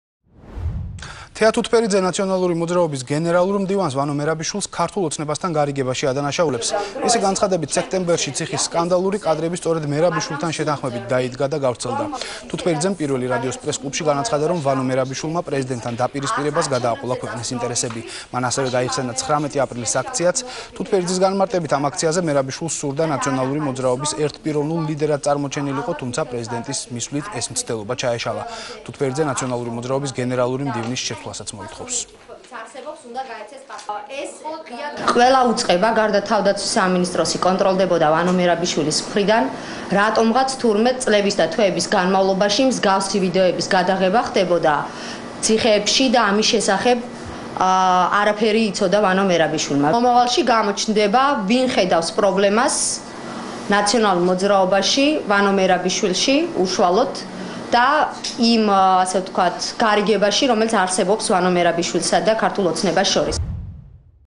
თეა თუთბერიძე ვანო მერბიშვილს ბრალდებებს უყენებს
თეა თუთბერიძე ე.წ. ციხის კადრების გავრცელებაში ვანო მერაბიშვილს ადანაშაულებს და აცხადებს, რომ "ქართულ ოცნებასთან" თანამშრომლობაში სდებს ბრალს.